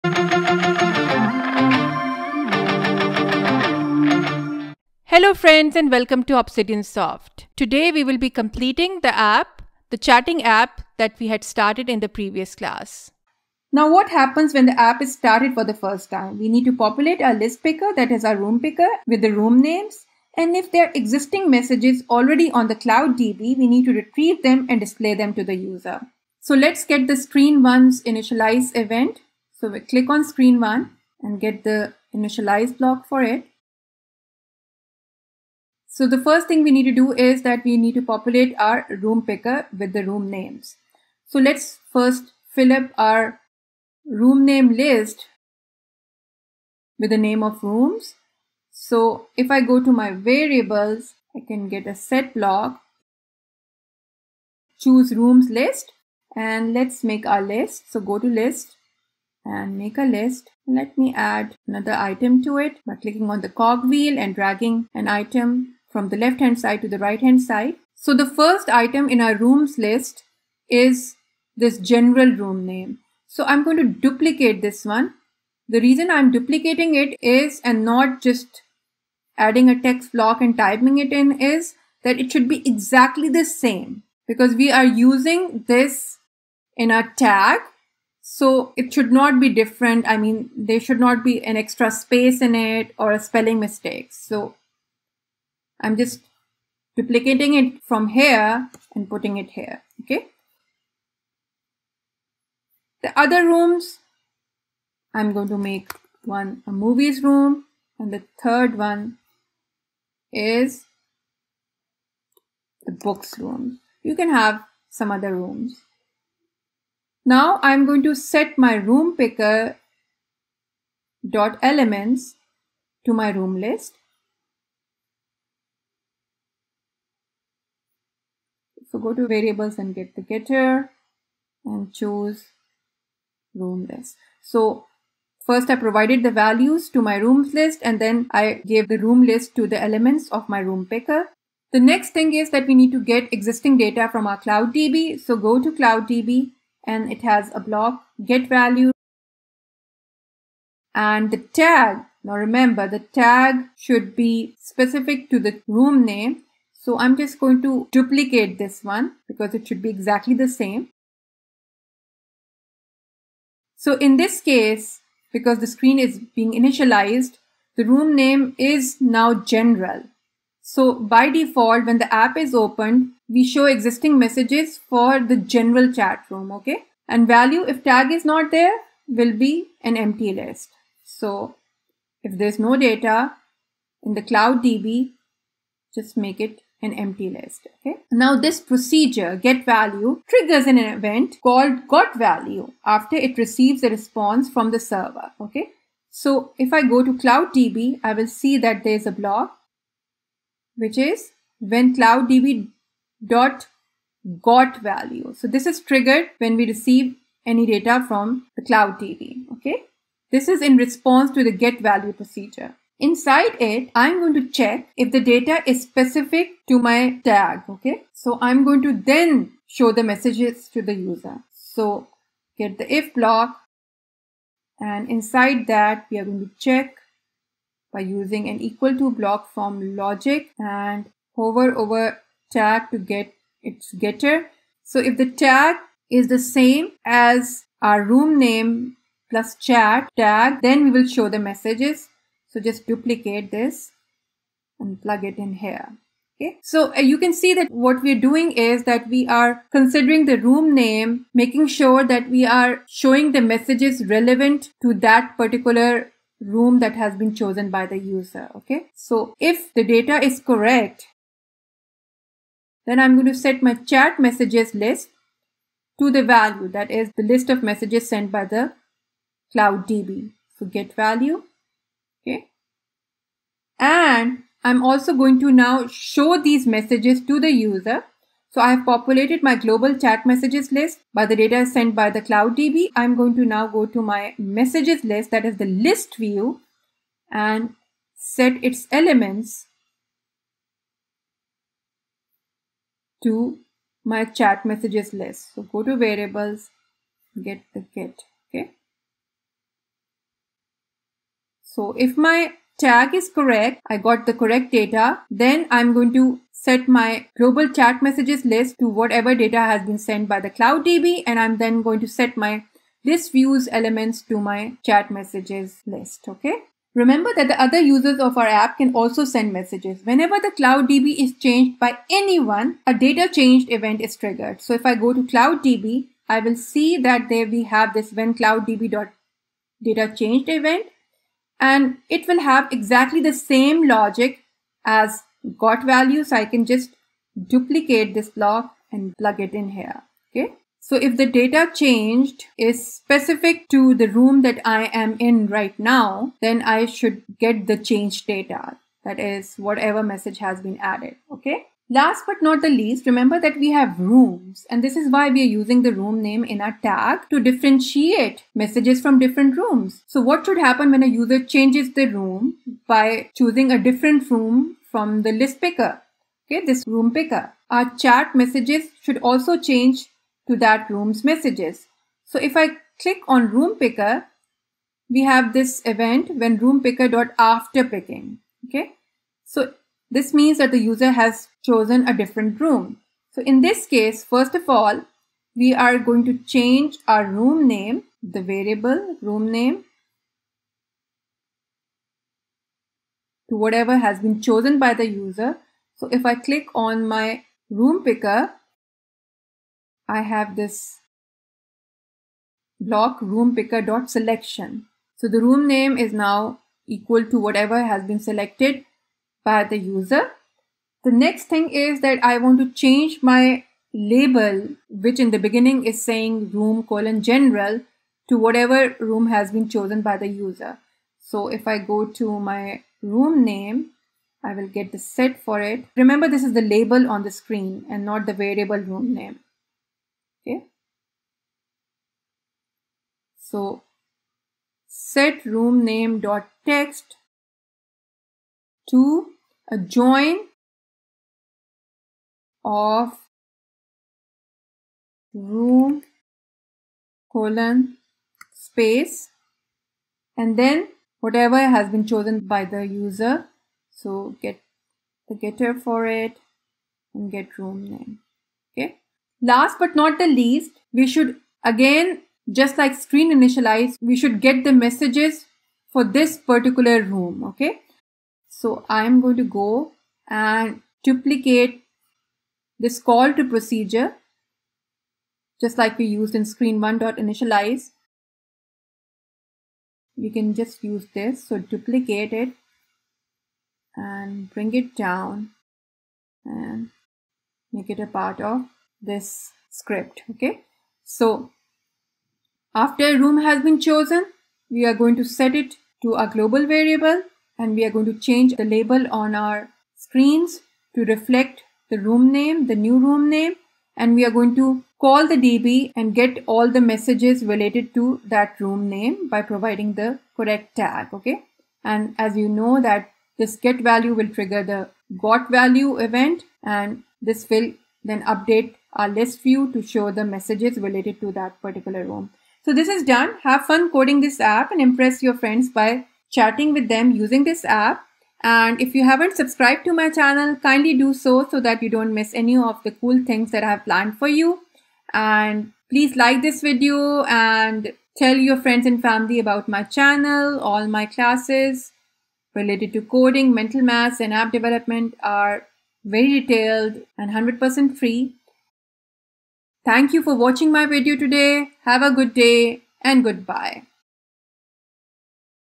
Chat App MIT App Inventor (Part 2)| Messenger App MIT App Inventor | CloudDB | Messaging app
In this video, I am teaching how to make a chat app in MIT App Inventor. Our chat app will also have rooms where users can chat on specific topics such as movies or books.
This is Part 2 of the Chat/Messenger App Tutorial for MIT App Inventor.

📣 📣 📣 VERY IMPORTANT 📣 📣 📣
You need two devices to test the app. At least one of the devices should have the app installed as two devices can't connect via MIT AI companion to the same project. So, install apk on one device and connect the second device through MIT AI companion app.
The first step is providing the user name and pressing send. Then, you can start sending messages.

Chapters:
00:00 Introduction
00:14 Screen initialization
00:35 Filling up ListPicker
02:21 Get existing messages from CloudDB
02:25 Using CloudDB GetValue
03:10 Using CloudDB GotValue
05:17 Using CloudDB DataChanged
05:50 Getting room specific chat messages

Special Thanks to Filmora for Intro Music

Please like my Facebook page and Instagram page for more FREE educational resources.